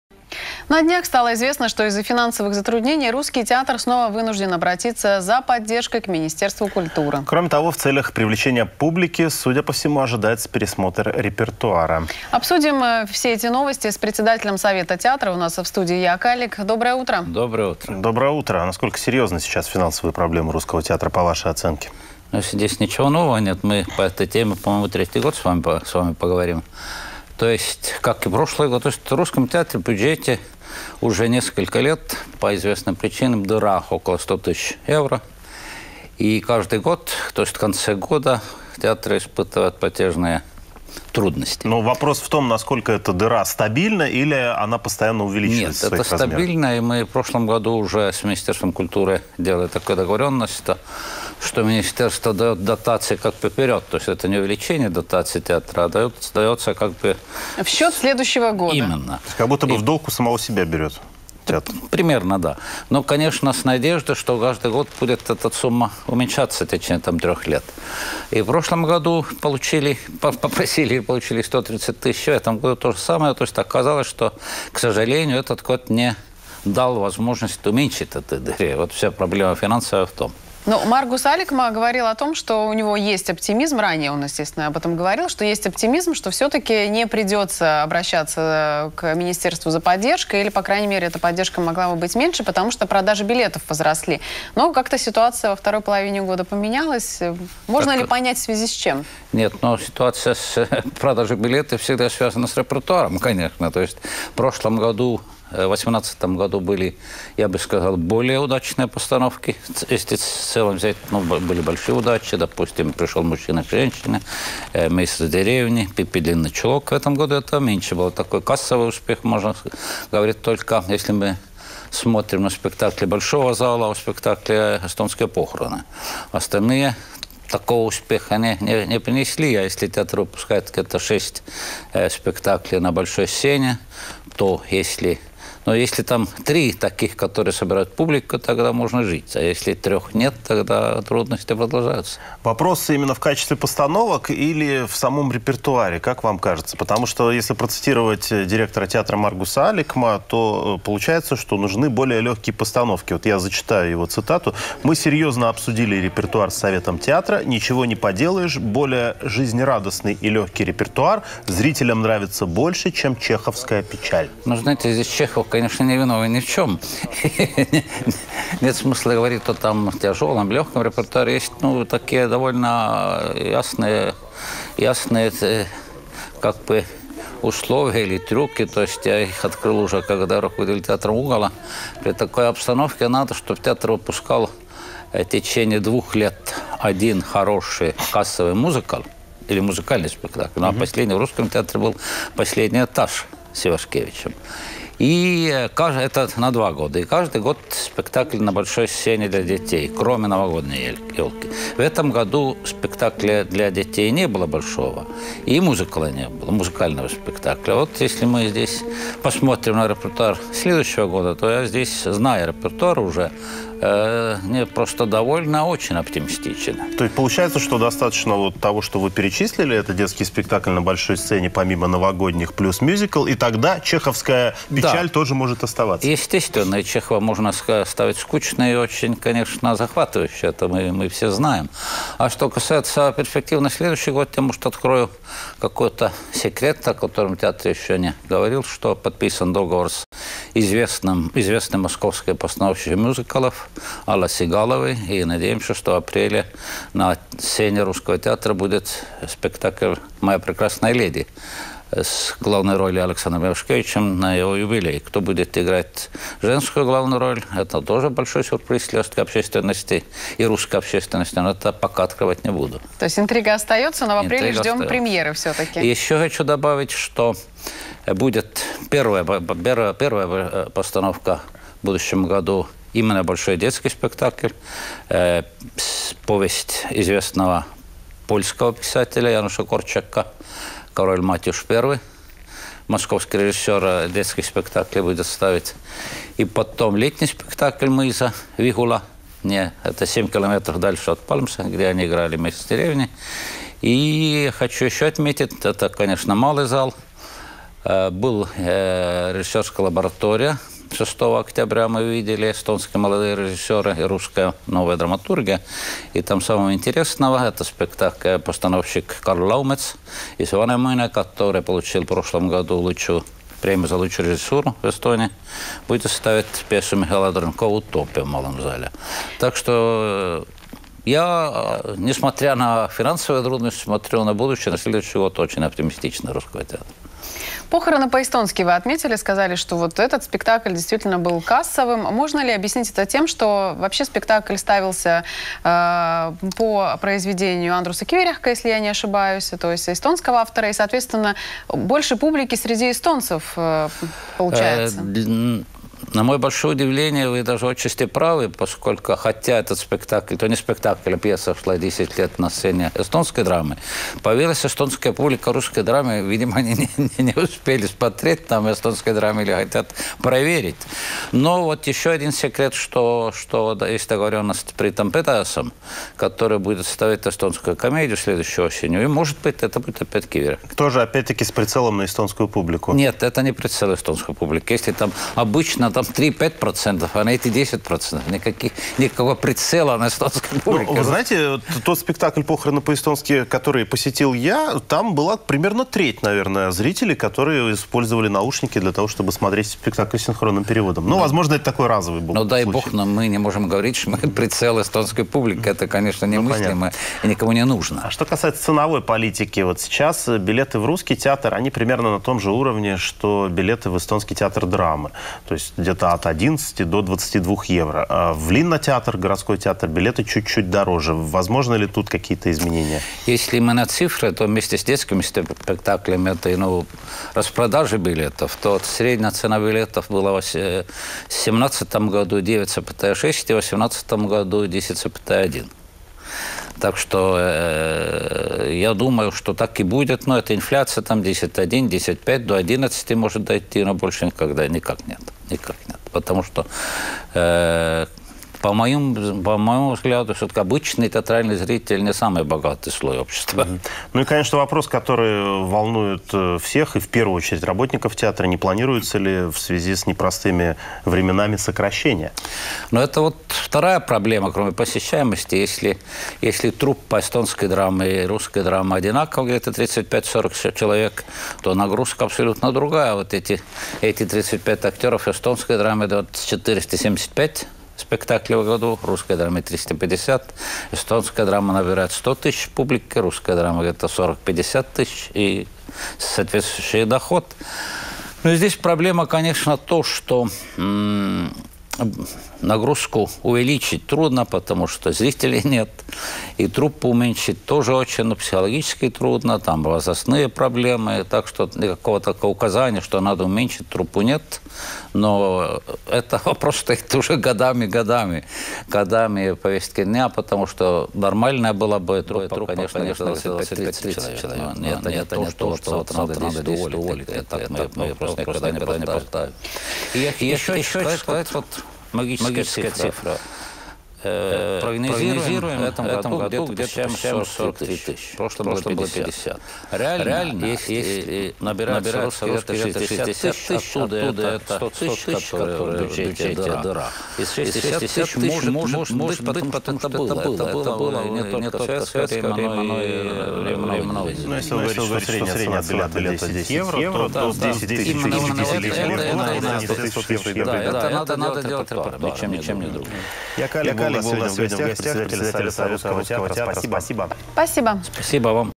Денег нет: Русский театр вынужден сменить репертуар, чтобы закрыть дыру в бюджете
На днях стало известно, что из-за финансовых затруднений Русский театр снова вынужден обратиться за поддержкой к Министерству культуры. Кроме того, в целях привлечения публики, судя по всему, ожидается пересмотр репертуара. У нас в студии председатель совета театра Яак Аллик.